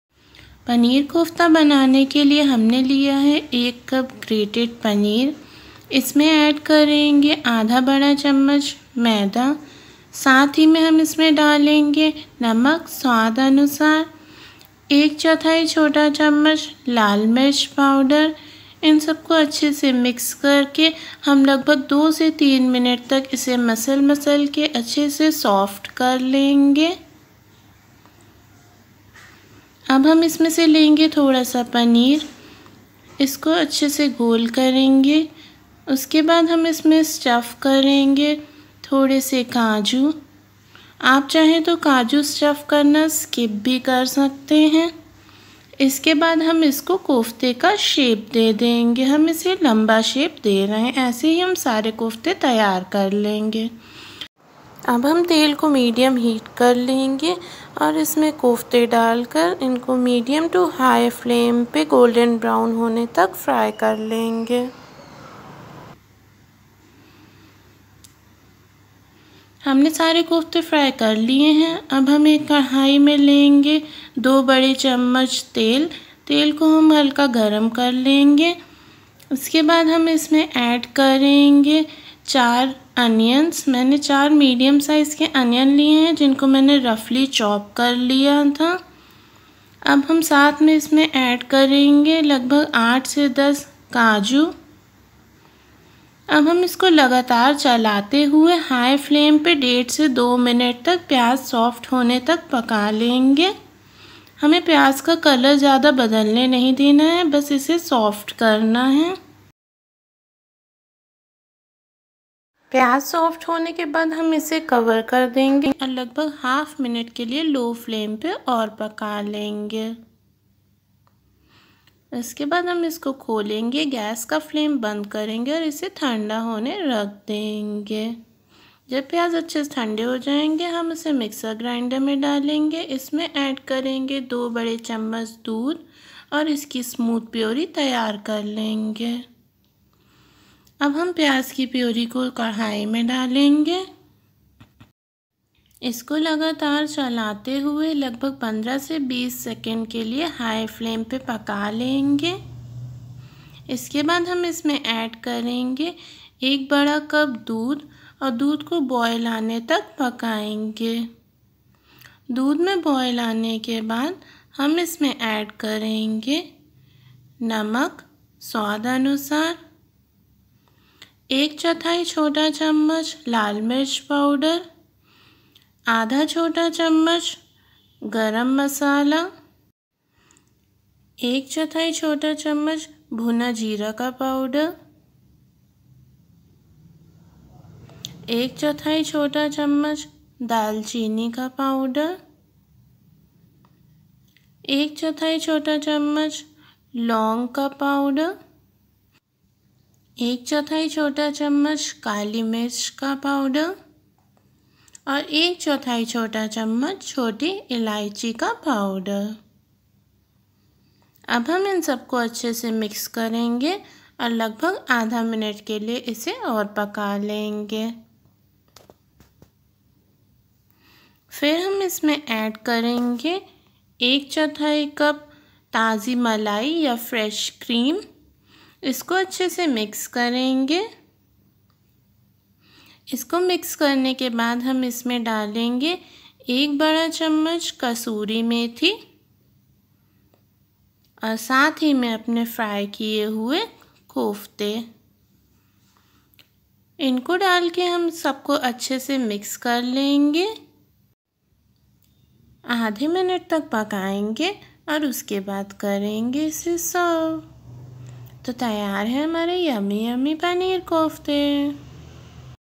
Restaurant style Paneer Malai kofta in white gravy|no ginger,garlic tomato|स्वादिष्ट पनीर मलाई कोफता
Veg Paneer Kofta

Ingredients
For Kofta's
Paneer- 1 cup grated
Refined Wheat Flour- 1/2 tablespoon
Cashew- for filling in kofta's
Oil- for frying
For Gravy
Onion- 4 medium
Cashew- 8-10
Milk- 1 cup + 2 tablespoon
Roasted Cumin Powder-1/4 teaspoon
Cinnamon Powder-1/4 teaspoon
Clove Powder-1/4 teaspoon
Black Pepper Powder-1/4 teaspoon
Green Cardamon Powder -1/4 teaspoon
Kasoori Methi- 1 tablespoon